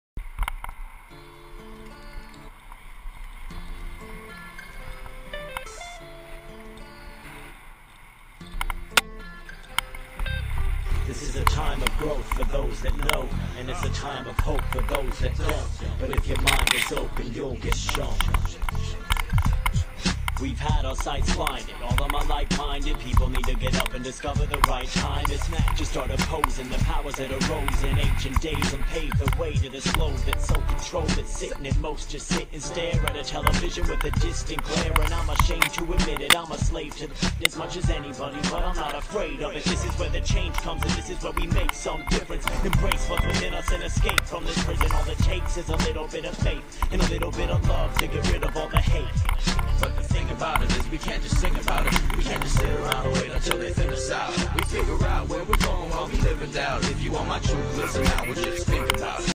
Chasing Joel Down Randa!
me chasing Joel Down top Part of Kuranda DH